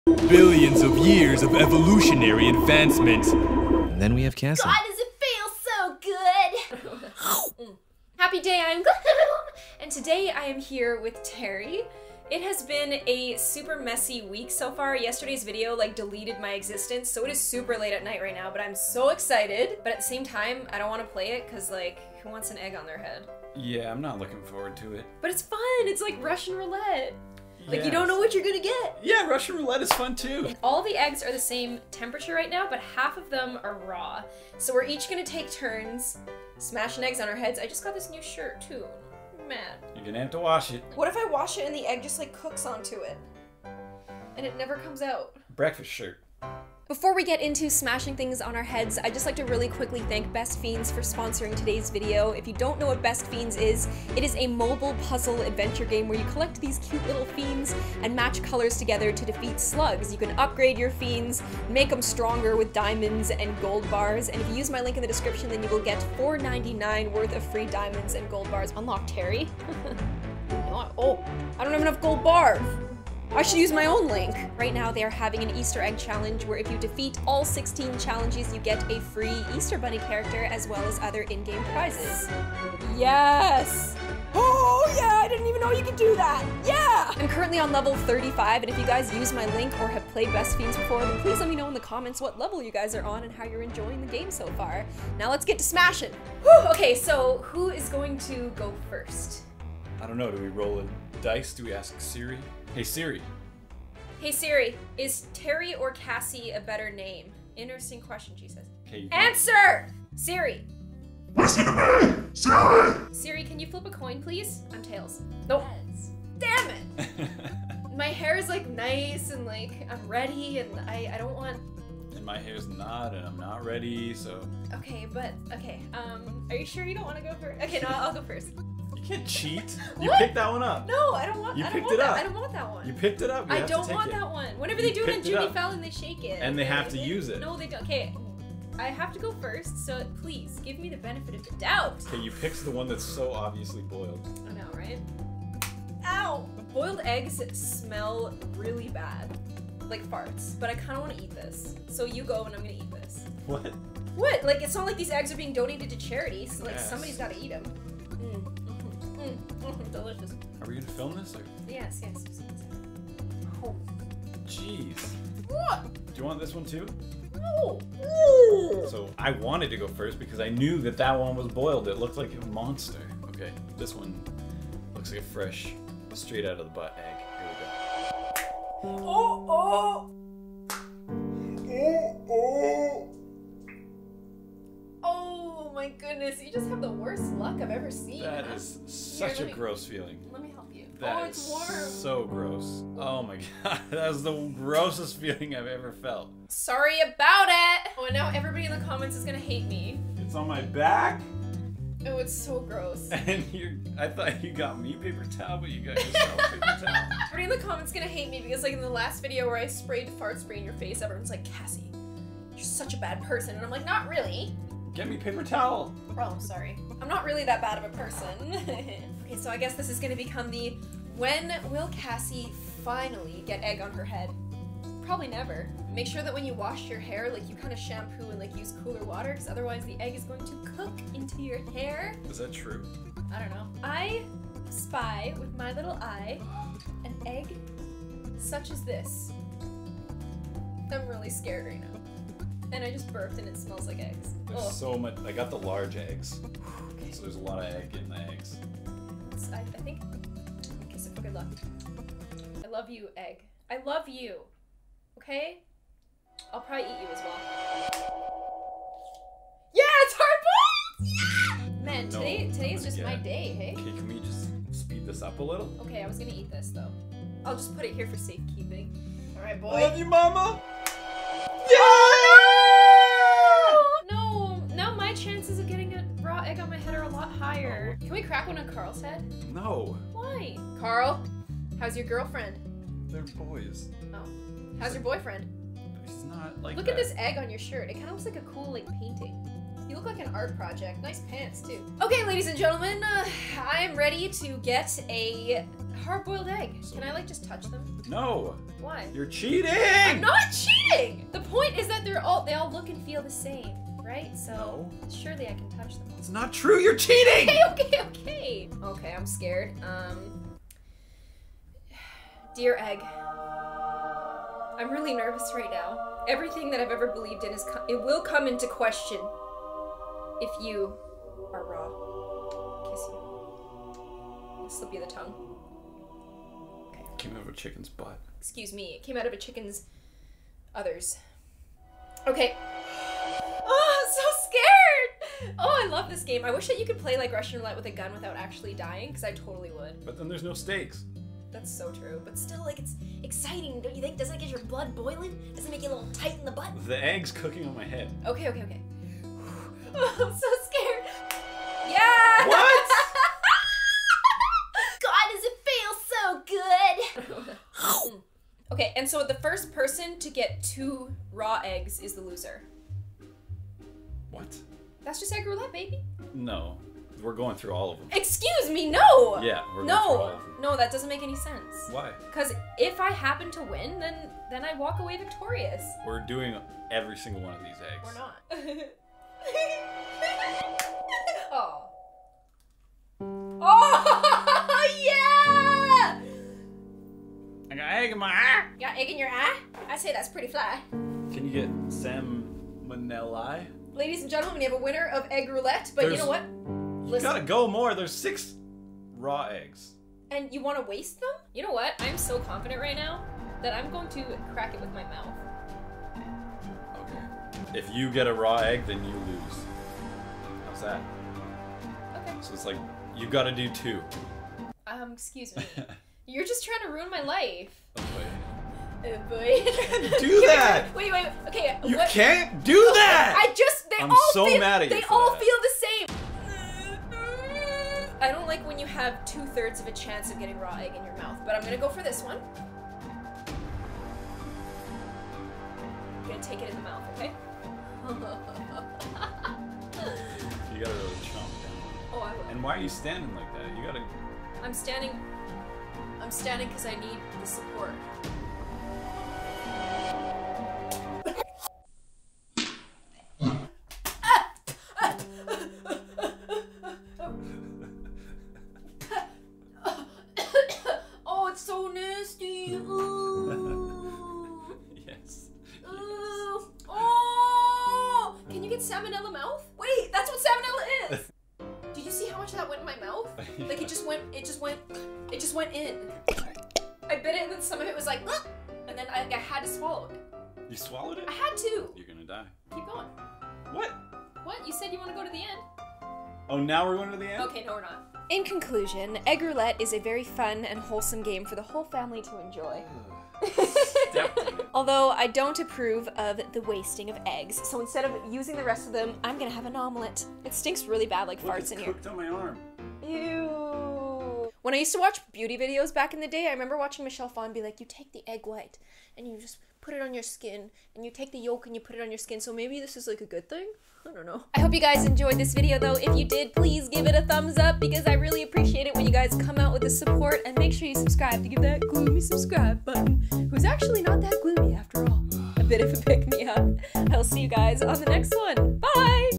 LOSER GETS SALMONELLA - Boyfriend Egg Roulette
Today Terry and I are playing a game I've always wanted to play-- Egg Roulette!